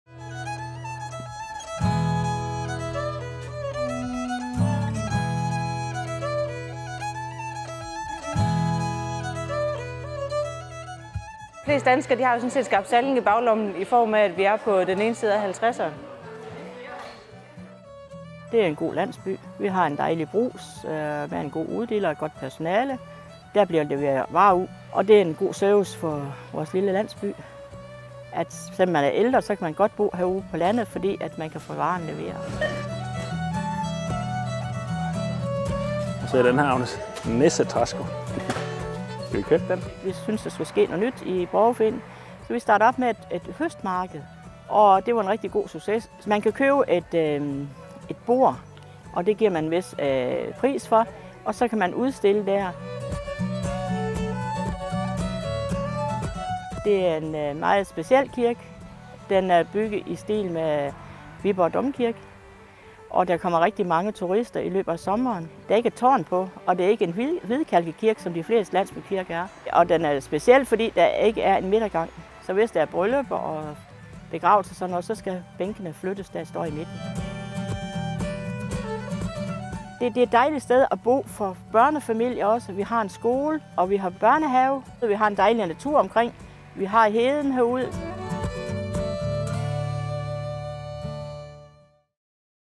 Meet the Locals: Skarp Salling
Skarp Salling har godt lokalsamfund med gode indkøbsmuligheder, en skole, børnehave, kirke og et forsamlingshus. Der bliver desuden afholdt fælles arrangementer såsom markeder.